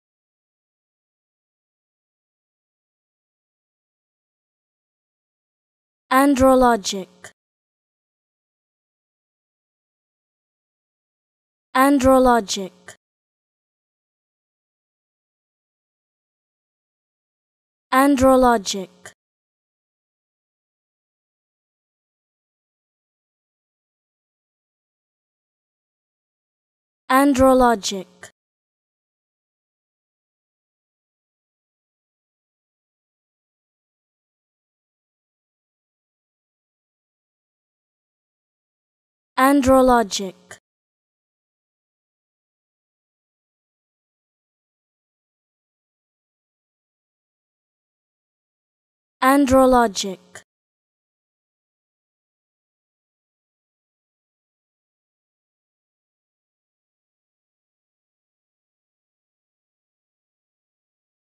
How To Say Andrologic
Learn how to say Andrologic with EmmaSaying free pronunciation tutorials.